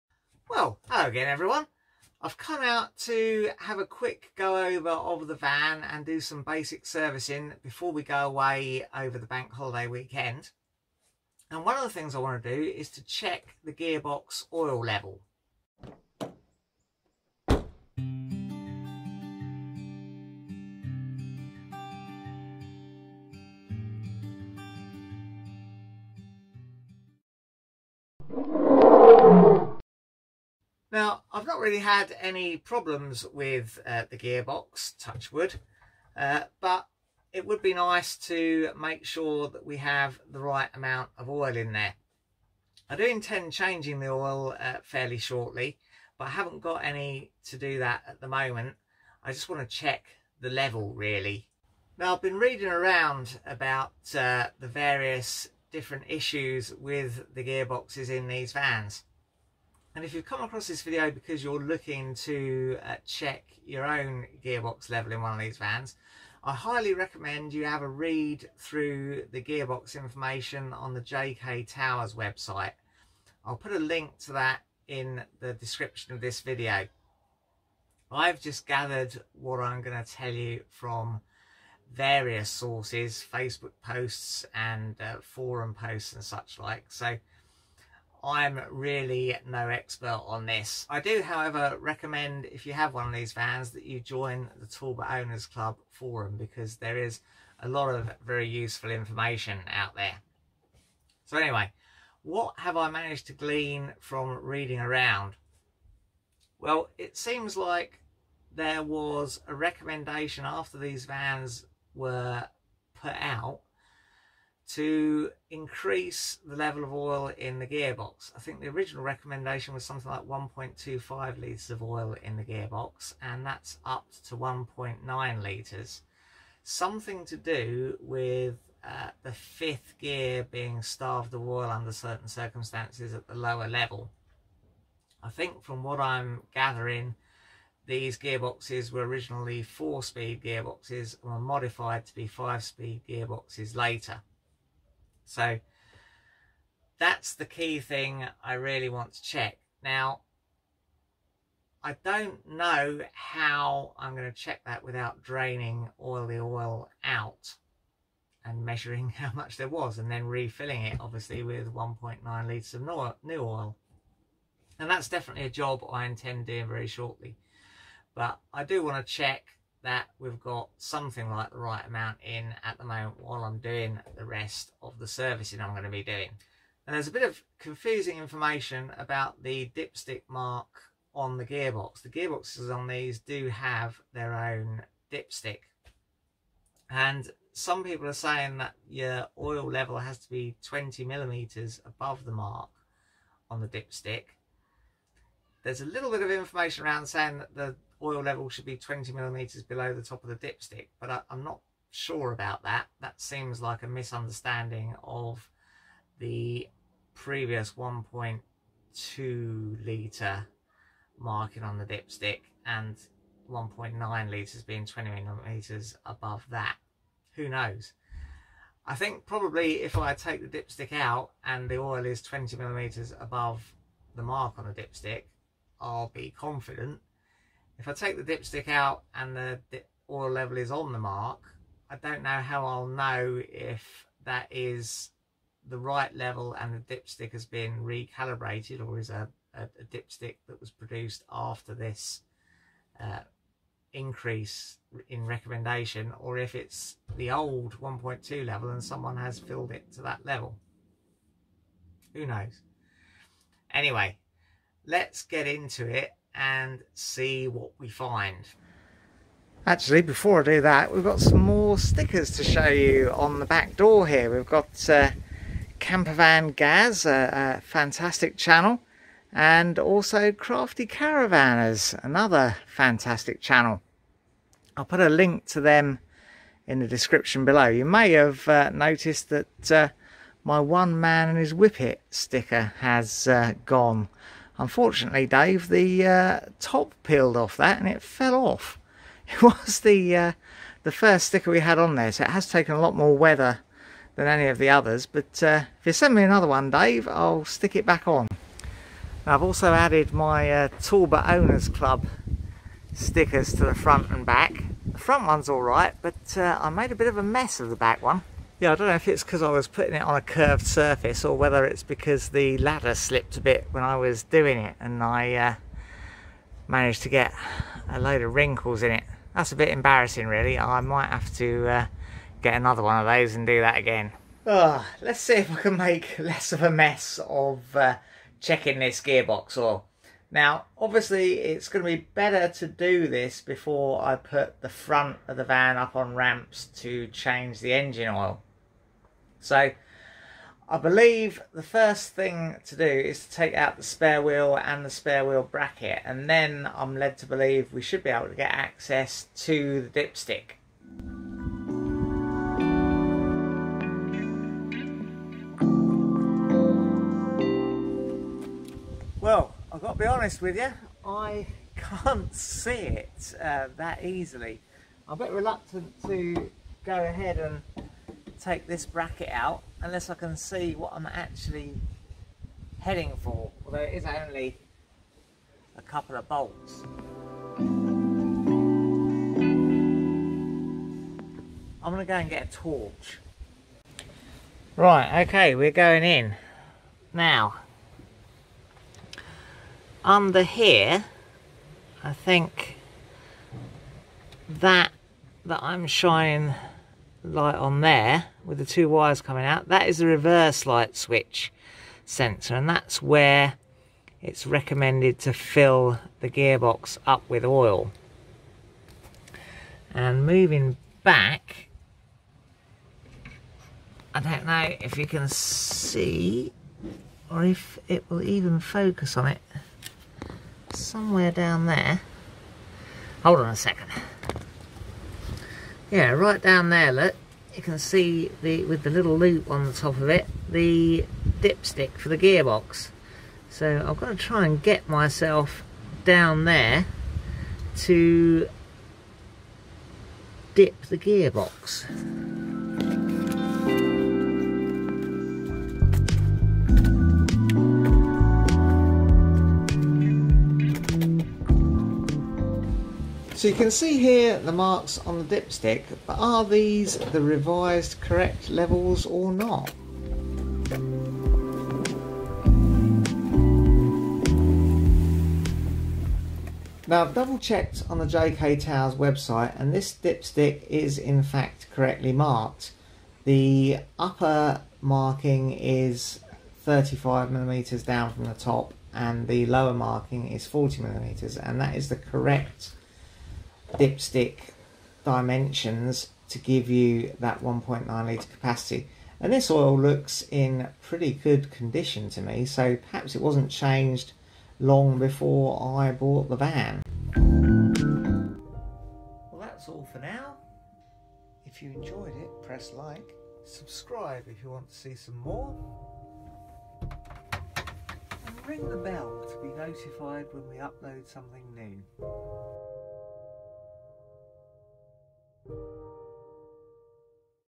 Checking The Gearbox Oil, Citroen C25 / Talbot Express / Peugeot J5 / Mk1 Fiat Ducato
Checking the gearbox oil level on my Citroen C25 motorhome (same as Talbot Express and Peugeot J5)

•Socials•